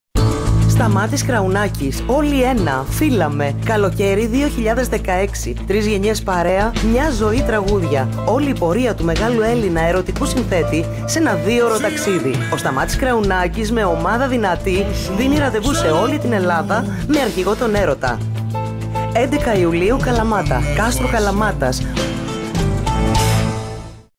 KRAOUNAKHS SPOT
Ο Σταμάτης Καρουνάκης στην Kαλαμάτα στις 11 Ιουλίου, στο Κάστρο: «ΟΛΟΙ ΕΝΑ!» φίλα με Τρεις γενιές τραγούδια!
Η δυναμική συμμετοχή και το ταλέντο των πέντε νέων καλλιτεχνών Χρήστου Γεροντίδη, Σάκη Καραθανάση, Κώστα Μπουγιώτη, Γιώργου Στιβανάκη, Χάρη Φλέουρα, του μαέστρου μας Άρη Βλάχου και των εξαιρετικών μουσικών μας Βάϊου Πράπα και Γιώργου Ταμιωλάκη, οι φωτισμοί του Κώστα Μπλουγουρά και το σκηνικό του Γιάννη Μουρίκη που μάγεψαν το κοινό, τα υπέροχα κοστούμια της Μαρίας Καραπούλιου, όλοι οι τεχνικοί και συνεργάτες, οι χορηγοί μας, όλοι μαζί, όλοι ένα, κατάφεραν αυτή η παράσταση να είναι μία μικρή νίκη Αγάπης. Όλοι ένα. Αρχηγός ο Έρωτας. Τρεις γενιές τραγούδια. Κοινή πορεία με τα αισθήματα και τα όνειρα των ζευγαριών, των μοναχικών, των ερωτευμένων. Όλα αυτά τα τραγούδια που μας κάνανε συνοδοιπόρους στα χρόνια και μου δώσανε διαβατήριο στις παρέες των νέων, στις συντροφιές, στα σπίτια των Ελλήνων. Ας πούμε σώζω στο σκληρό δίσκο εκείνα που θα ‘παιρνα μαζί μου. Τα απαραίτητα για ένα παγκόσμιο ταξίδι.. Επιλογή από λατρεμένα αλλά και κρυφά ενωμένα με ρετσιτατίβα και σπουδαία μικρά ποιητικά κείμενα βαφτισμένα στη μουσική που δε σταματάει ποτέ. Σουΐτα αισθημάτων, λειτουργικό της Αγάπης.. Τόσα βράδια μια ζωή.. Για δύο ώρες στο πιάνο μου και παντού, ανάμεσά σας, με καλεσμένους τρεις θαυμάσιους συνεργάτες, Άρης Βλάχος μουσική διεύθυνση-πλήκτρα, Βάϊος Πράπας λαϊκά έγχορδα-κιθάρα, Γιώργος Ταμιωλάκης βιολοντσέλο και πέντε αγγέλους συμπαραστάτες, δυνατούς καλλιτέχνες και δοσμένους στην τέχνη τους ολόψυχα. Αλφαβητικά: Χρήστος Γεροντίδης, Σάκης Καραθανάσης, Κώστας Μπουγιώτης, Γιώργος Στιβανάκης και ο Χάρης Φλέουρας. «Ότι και να γίνει, όσο και μοντέρνοι να μας νοιάζει να γίνουμε, αυτό που μας ενώνει με τη ρίζα ενός κινήματος, είναι αυτό που χωρίς να το επιδιώξουμε… κρυφά και ταπεινά μας ένωσε με τα όνειρα των διπλανών μας…»
ΣΥΝΤΕΛΕΣΤΕΣ Σκηνικό : Γιάννης Μουρίκης Φωτισμοί : Κώστας Μπλουγουράς Κοστούμια : Μαρία Καραπούλιου Ήχος : Δημήτρης Σωτηρόπουλος + Κώστας Χριστοφής Βοηθός σκηνοθέτη: Χρήστος Μουστάκας Παραγωγή : Ομάδα Ελληνικού Μουσικού Θεάτρου –Σπείρα Σπείρα Οργάνωση παραγωγής : Ελένη Συροπούλου Προβολή & Επικοινωνία : Δέσποινα Κραουνάκη Σχεδιασμός προγράμματος : Χρήστος Μουστάκας
11 ΙΟΥΛΙΟΥ - ΑΜΦΙΘΕΑΤΡΟ ΚΑΣΤΡΟΥ ΚΑΛΑΜΑΤΑΣ: ΩΡΑ ΕΝΑΡΞΗΣ 21.15